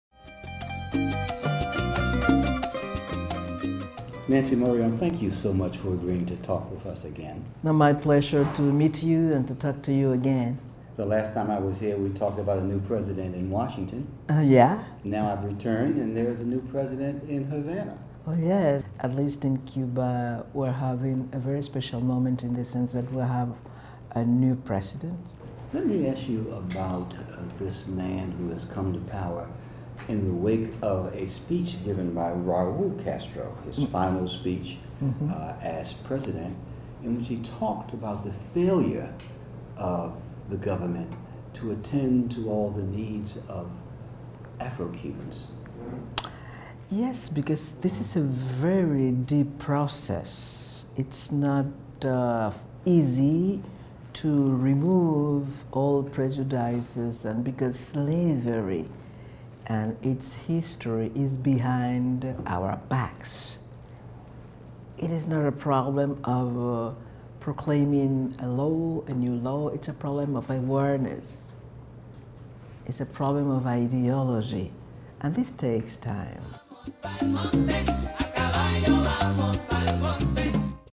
SGJC Dean Dewayne Wickham Interviews Cuba Poet Nancy Morejon
This interview is part of a documentary project that will examine the role of Cuban artists and how their arts are influencing and impacting the lives of Afro Cubans.